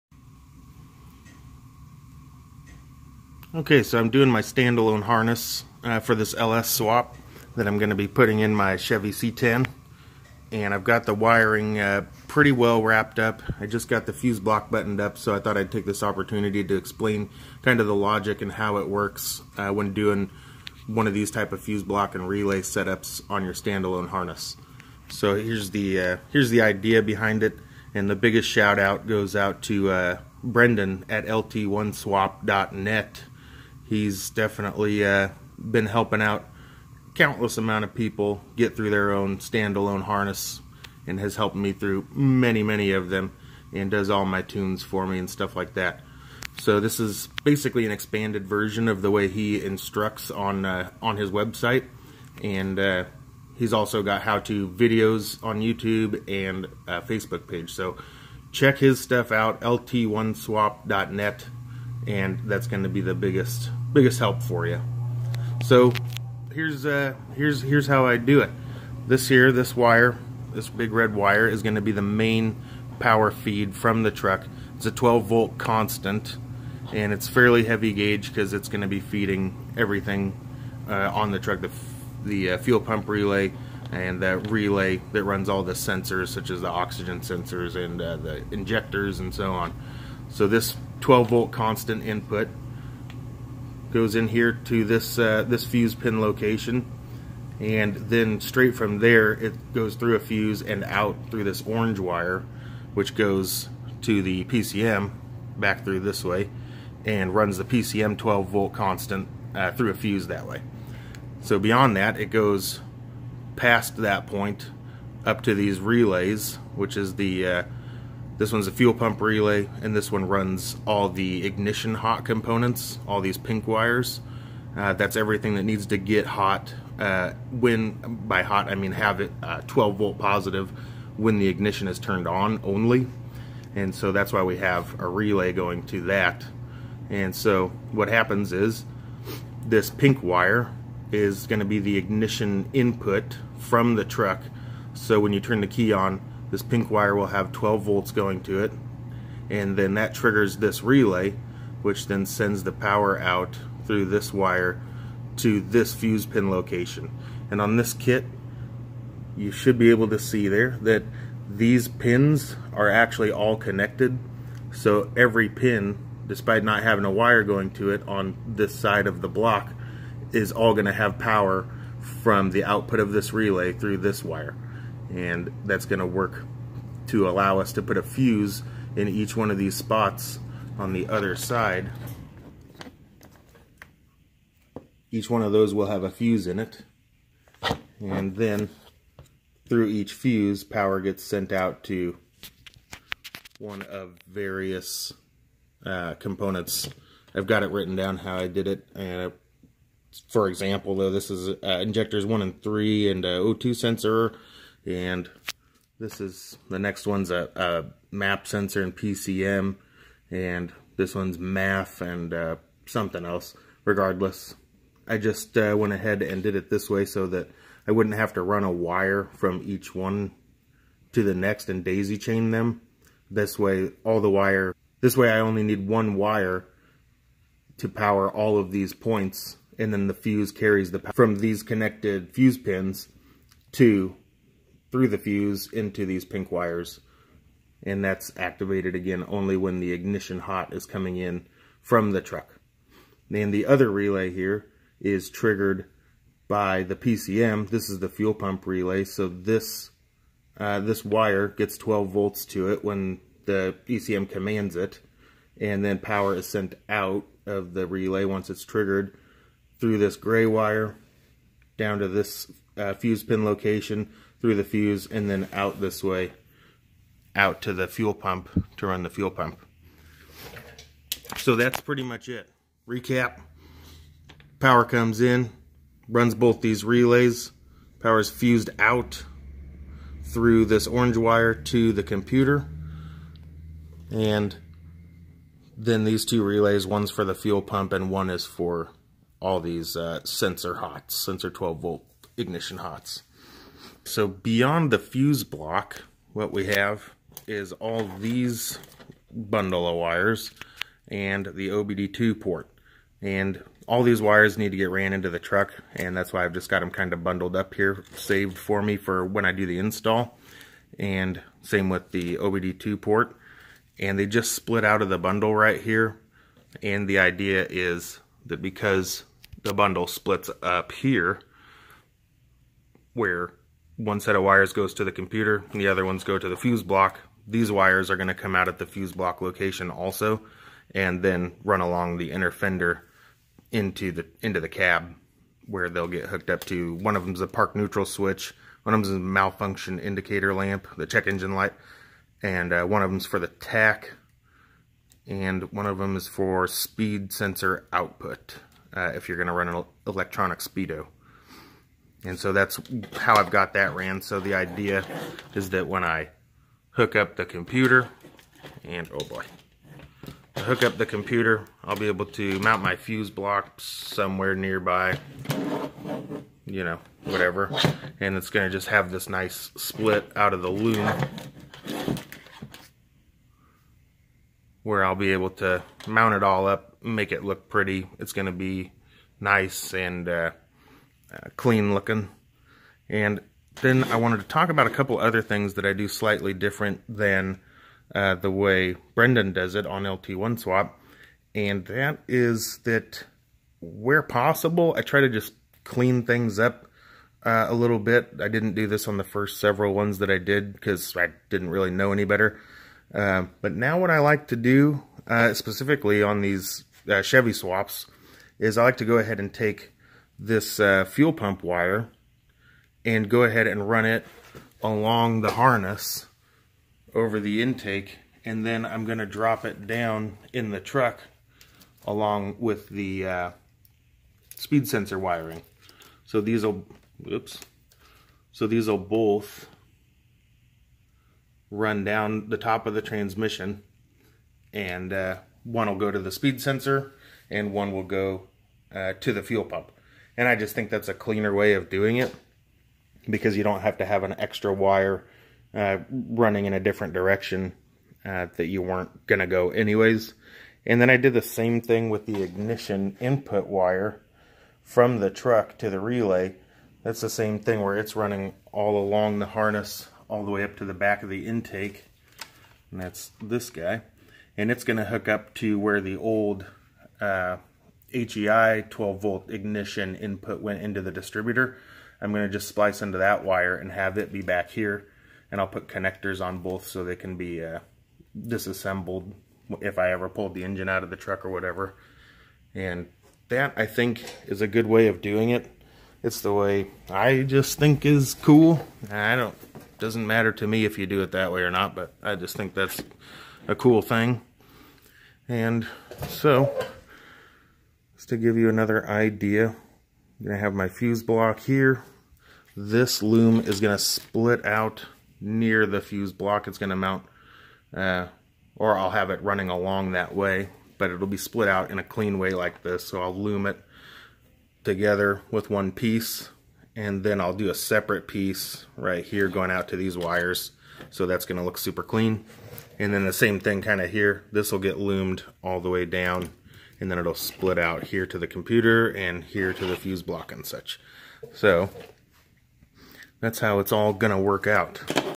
Stand alone ls swap harness fuse block logic.
The biggest credit for my knowledge of this subject goes out to Brendan at lt1swap.com! Check out his site, his Facebook page, and his YouTube channel! This video just talks about the ways I have used my experience with his priceless information, and modified it to fit my personal preference.

Fuse block,

OBD2 port,

Basic wire crimpers,

I do offer LS swap stand alone wire harness rework service. I even sell complete engine packages.